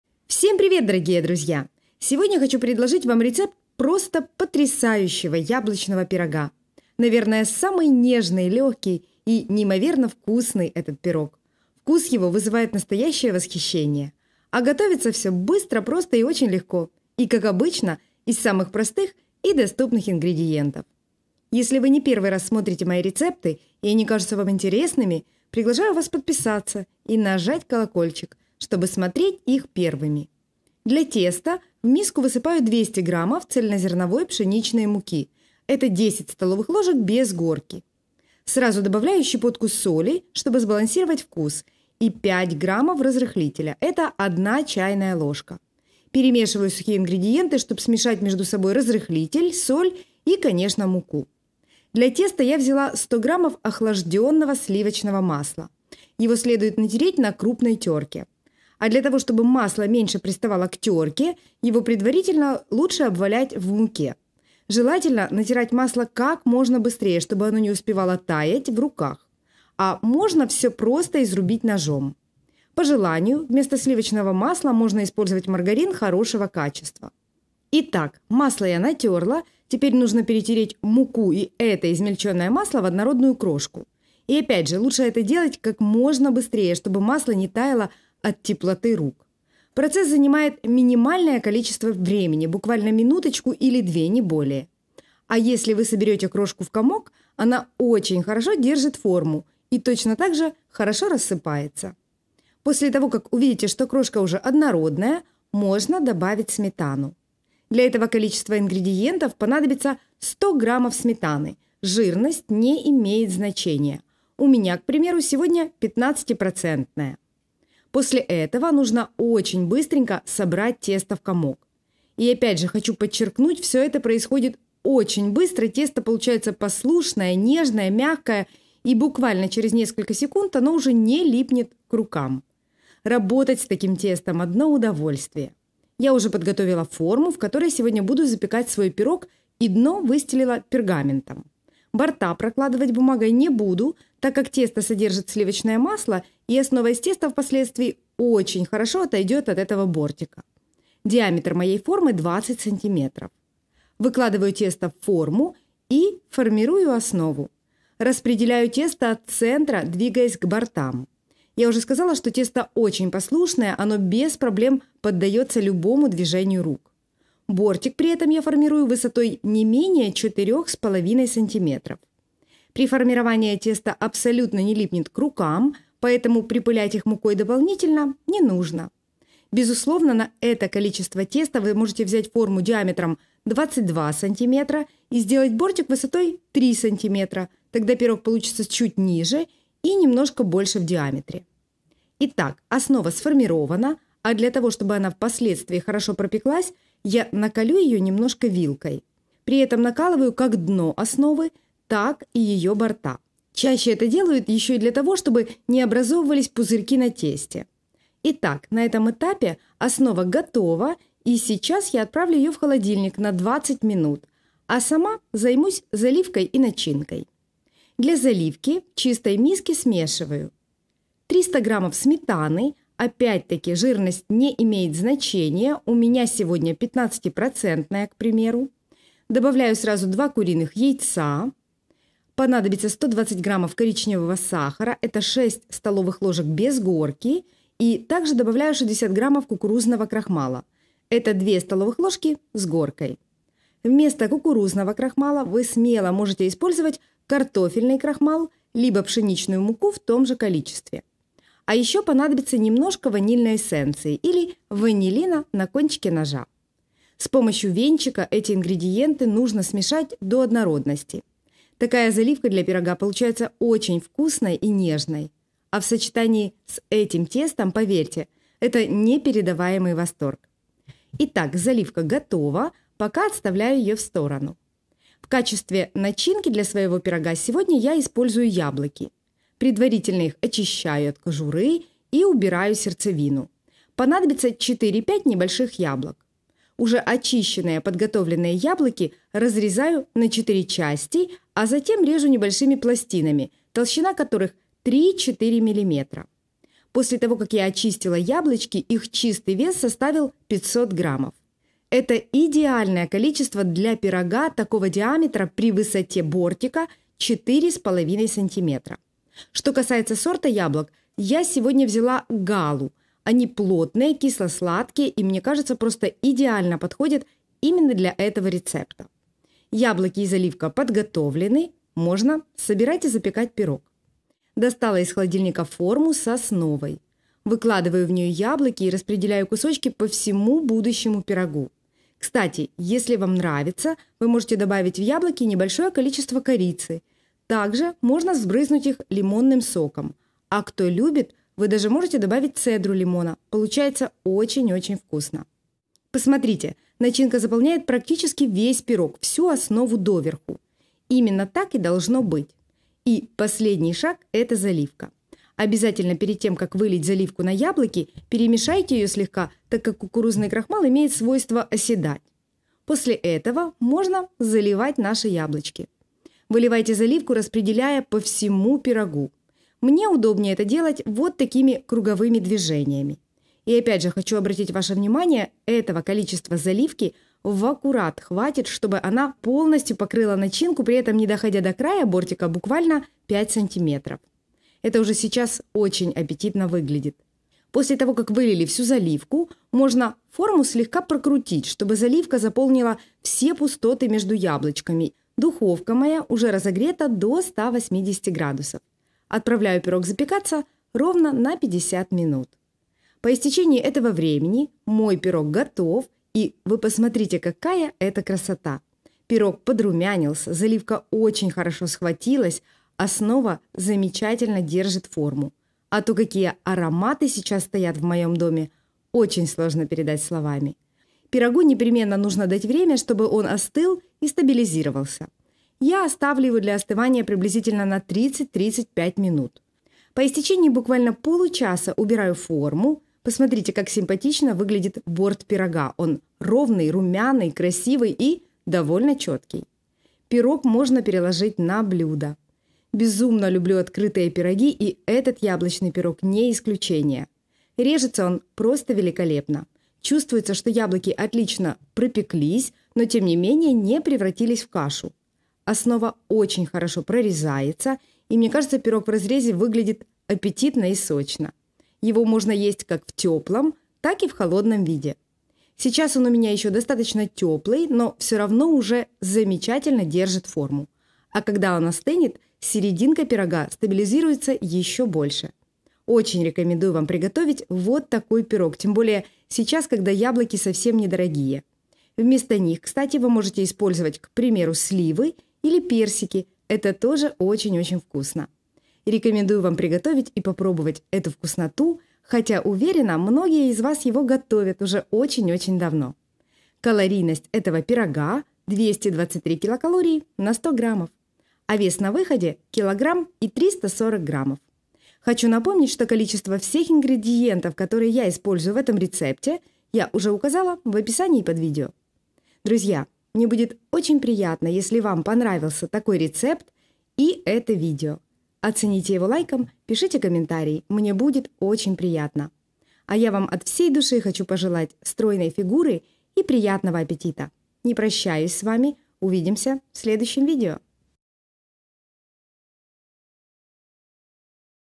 ВЫПРОСИЛА РЕЦЕПТ У СВЕКРОВИ. Теперь ЯБЛОЧНЫЙ ПИРОГ готовлю только так!!!
Наверное самый нежный лёгкий и неимоверно вкусный пирог.
Вкус  его  вызывает настоящее восхищение.
А готовиться всё быстро просто и очень легко, и как обычно из самых простых и доступных ингредиентов.

ЯБЛОЧНЫЙ ПИРОГ
КБЖУ НА 100 ГР:
223/3,7/11,0/28,0
Диаметр моей формы 20 см
Высота бортика формы 5 см
Вес пирога на выходе 1,34 кг

НАМ ПОНАДОБИТСЯ
ДЛЯ ОСНОВЫ:
Мука цельнозерновая пшеничная 200 гр (10 ст.л без горки)
Сметана (у меня 15%жирности) 100 гр
Соль 1 щепотка
Разрыхлитель 1 ч.л (5 гр)

ДЛЯ ЗАЛИВКИ:
Сметана (у меня 15%жирности) 300 гр
Яйца 2 шт (110 гр)
Сахар коричневый 120 гр (6 ст.л без горки)
Кукурузный крахмал 60 гр (2 ст.л с горкой)
Ванильная эссенция 5 капель

ДЛЯ НАЧИНКИ:
Яблоки 4-5 шт (500 гр в очищенном виде)

ЗАПЕКАТЬ В ДУХОВКЕ ПРИ 180 ГРАДУСАХ 50 МИНУТ.

Это видео снято на камеру PANASONIC DC-GH5